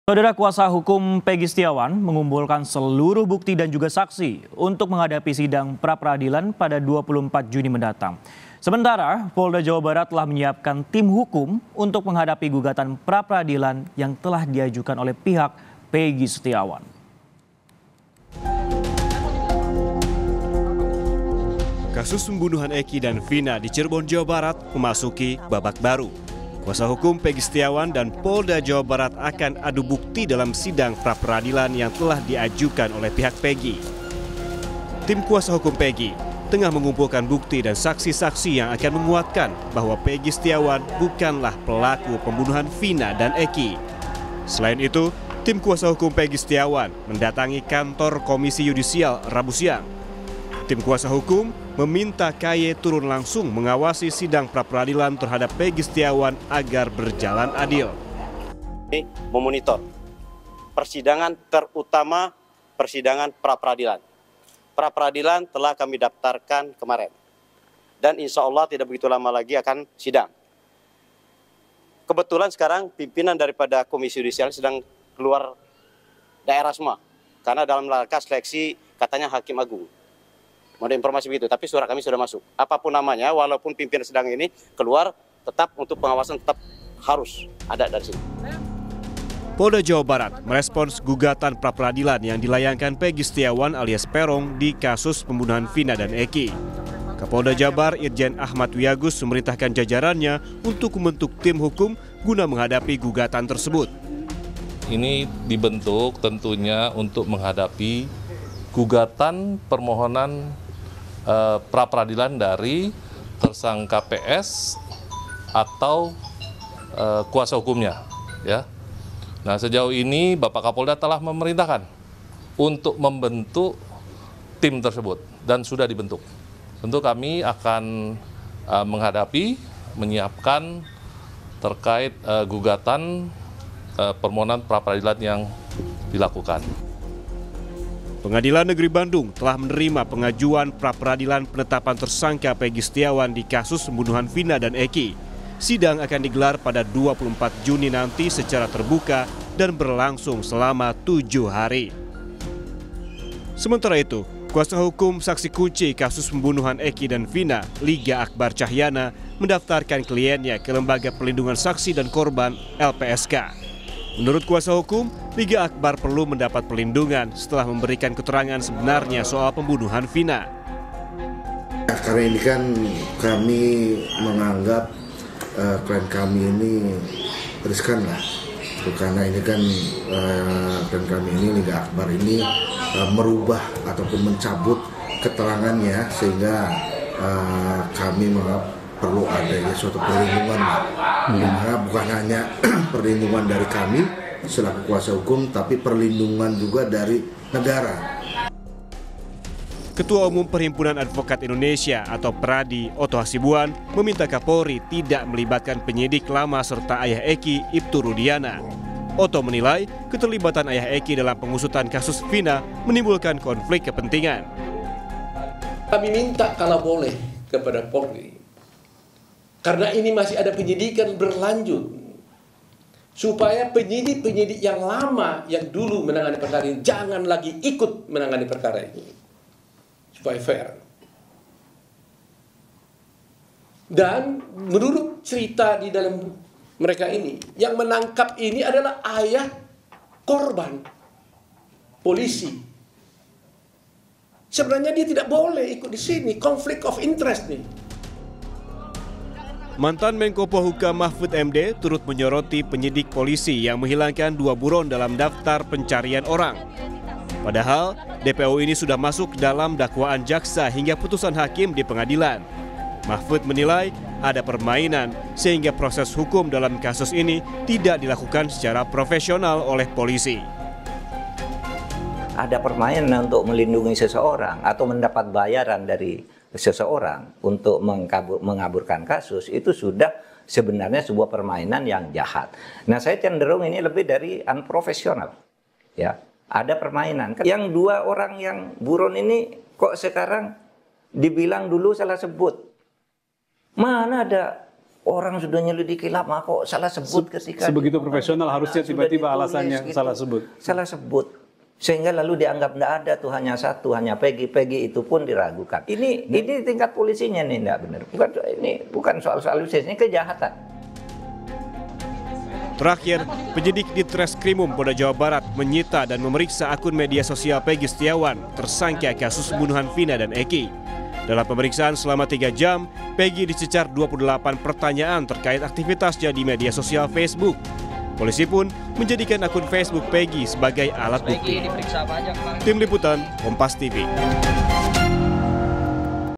Update Seputar Kasus Vina Cirebon: Pegi Segera Jalani Praperadilan, Liga Akbar Minta Perlindungan
KOMPAS.TV - Kuasa Hukum Pegi Setiawan, mengumpulkan seluruh bukti dan saksi untuk menghadapi sidang pra-peradilan, 24 Juni mendatang.

Sementara Polda Jawa Barat telah menyiapkan tim hukum untuk menghadapi gugatan pra-peradilan yang telah diajukan pihak Pegi Setiawan.

Kasus pembunuhan Eky dan Vina di Cirebon, Jawa Barat, memasuki babak baru. Kuasa Hukum Pegi Setiawan dan Polda Jawa Barat akan adu bukti dalam sidang praperadilan yang telah diajukan oleh pihak Pegi.

Baca Juga Jelang Praperadilan di Kasus Vina Cirebon, Jejak Digital Pegi Akan Dibawa Pengacara dan Polda Jabar di